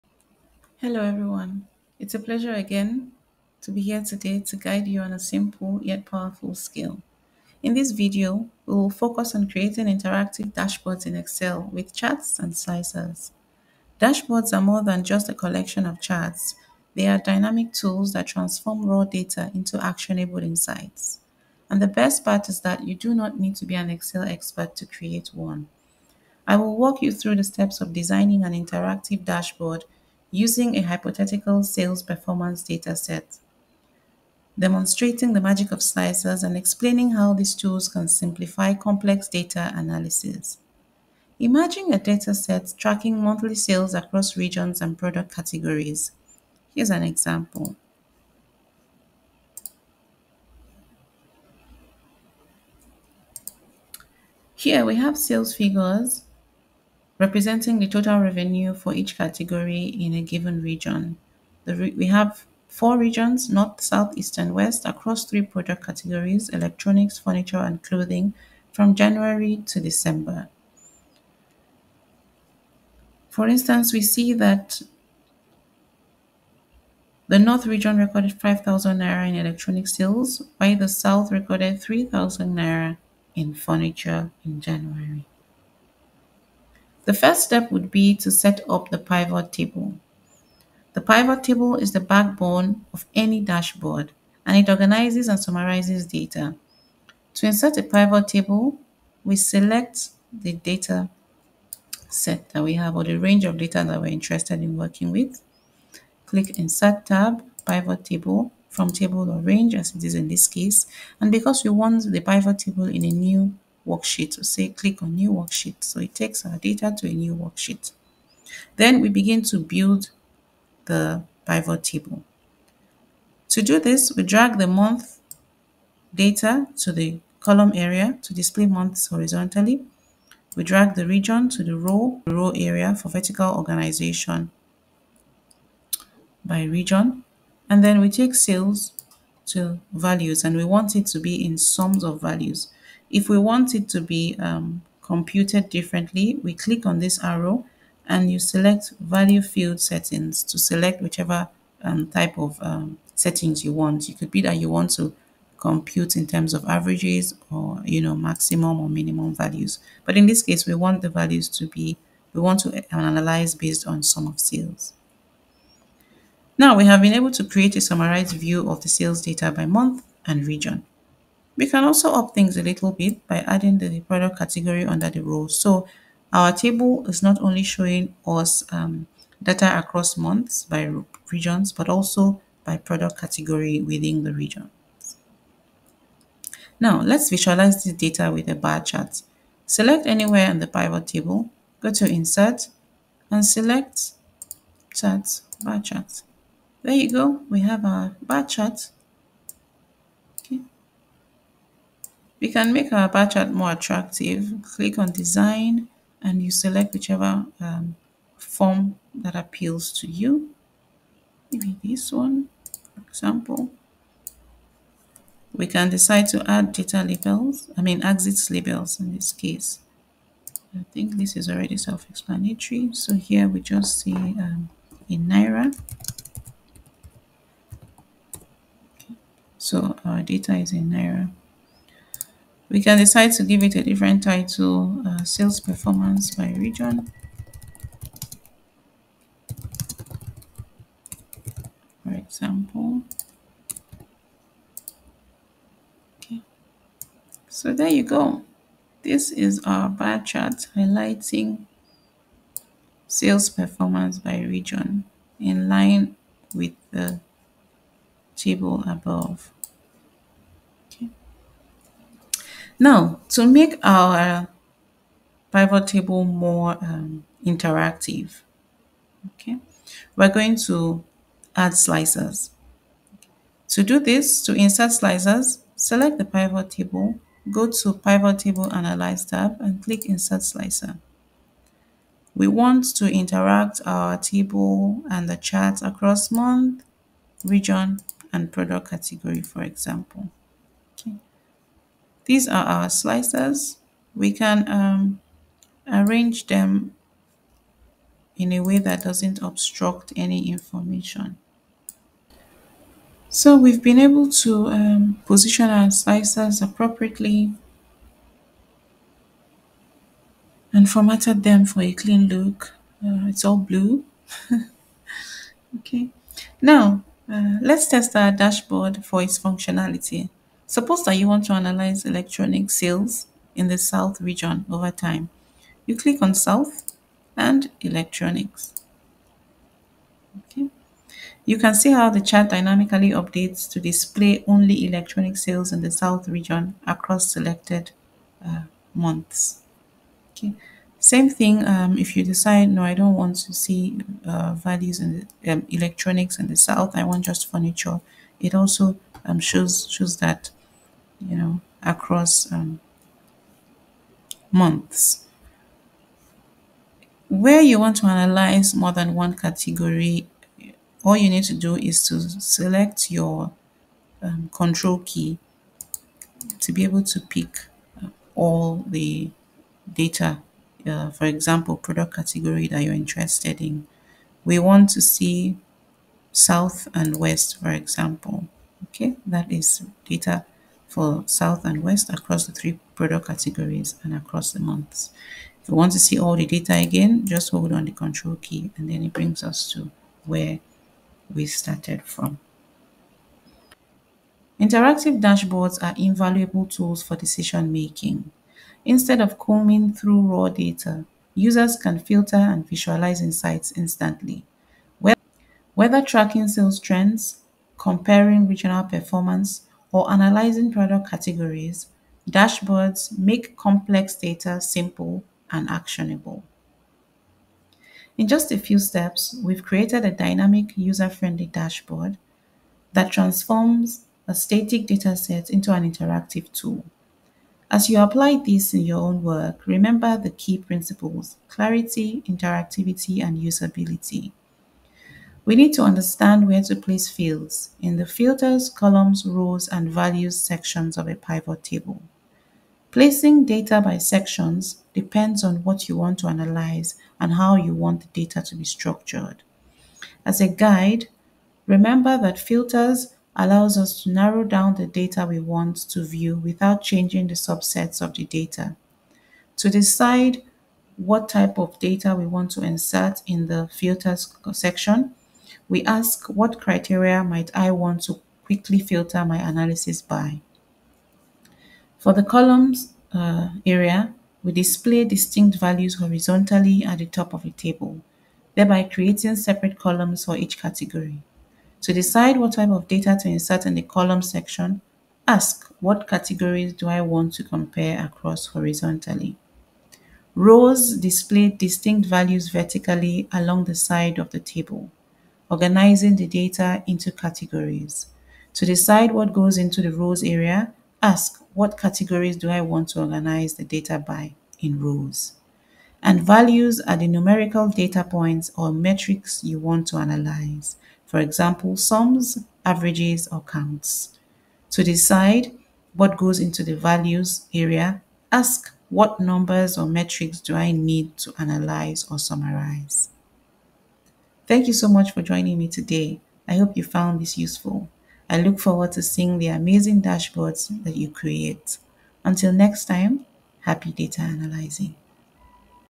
Creating Interactive Dashboards in Excel
Transform your data analysis skills with this simple tutorial!

In this video, I’ll show you how to create a professional interactive dashboard in Excel using a hypothetical sales dataset. Learn to:
1️⃣ Build PivotTables for quick data summaries
2️⃣ Create and format dynamic charts
3️⃣ Use slicers for easy, interactive filtering

This tutorial is perfect for beginners and professionals looking to simplify their reporting.

🌟 Ready to level up your Excel game? Watch now